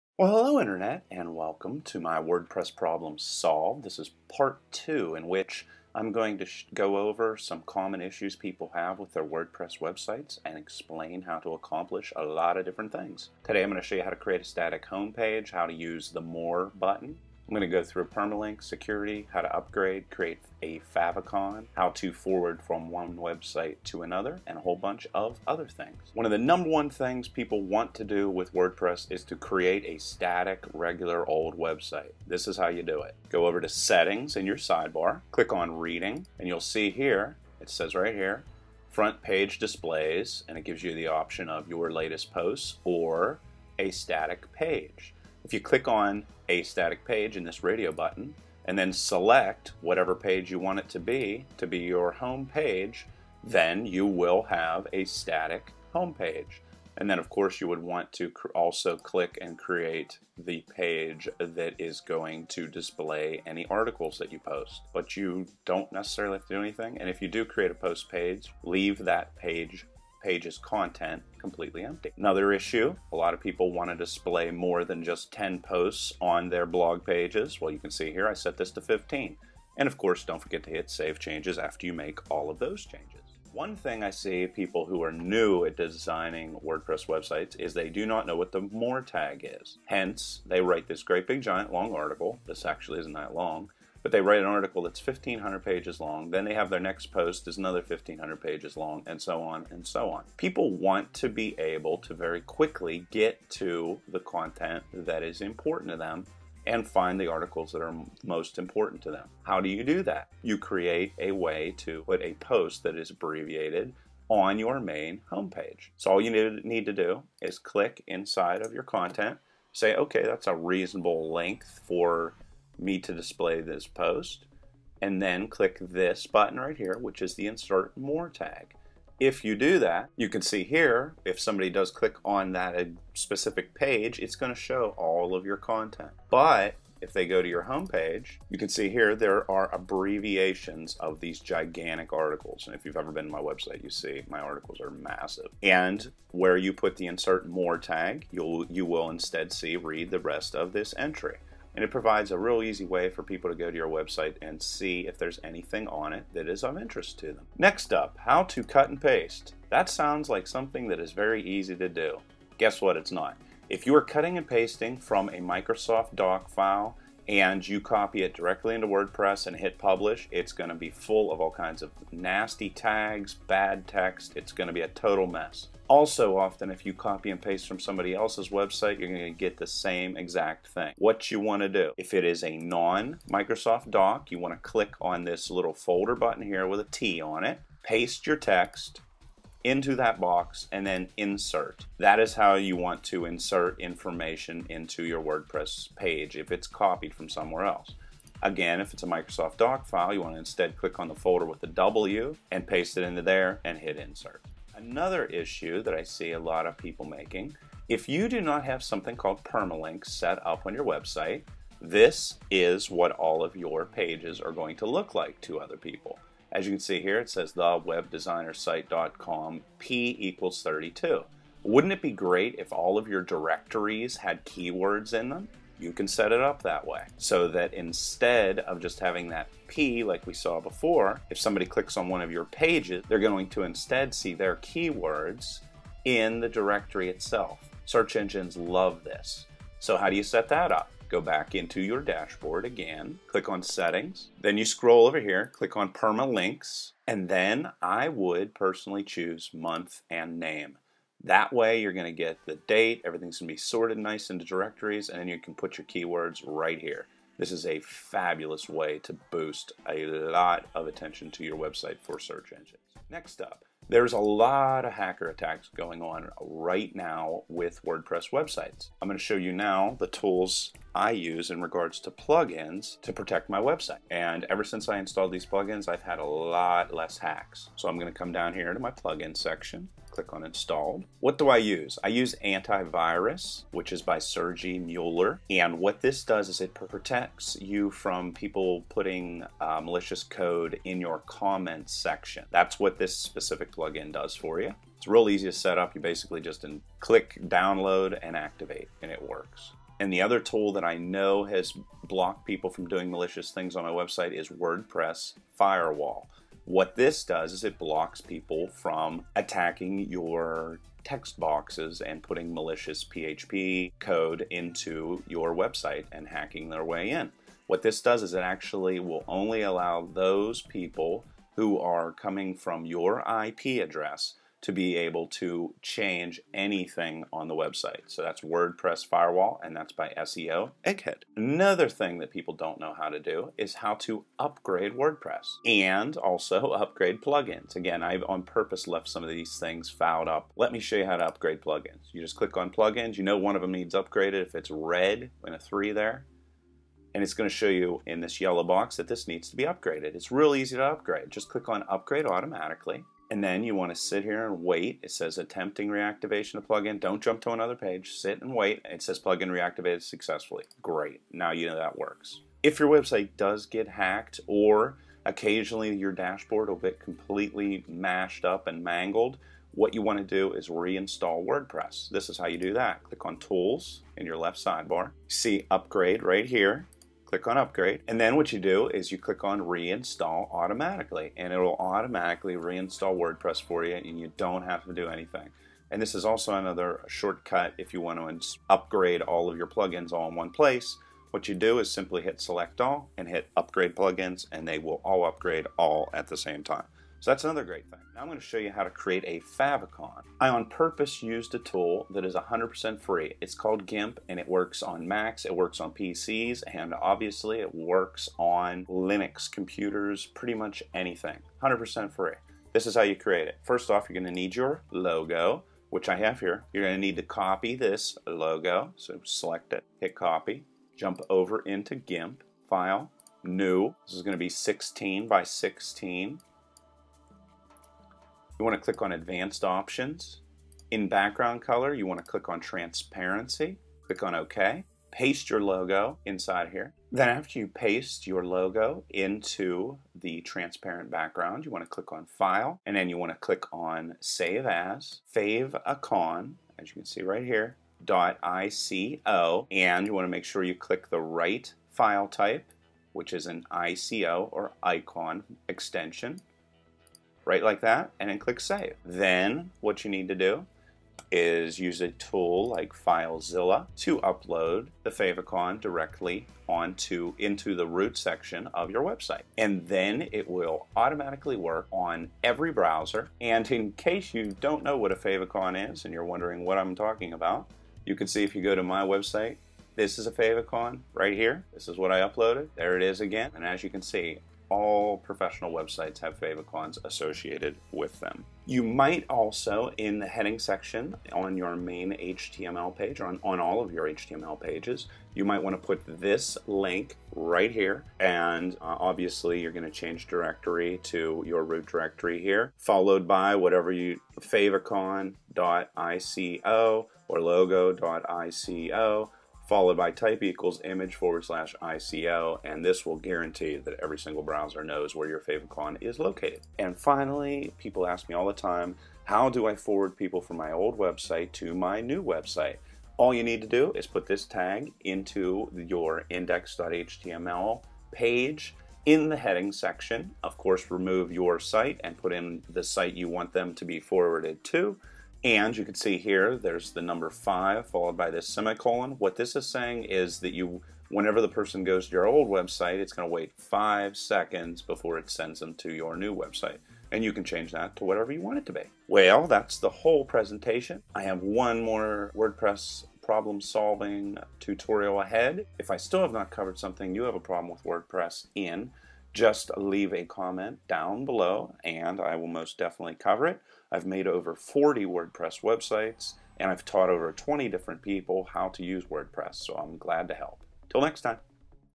Wordpress Problems Solved Pt 2
Here I show you how to solve common Wordpress problem's. How to: static home page, more tag, permalinks, security wordpress plugins, Make a Favicon, forward to other site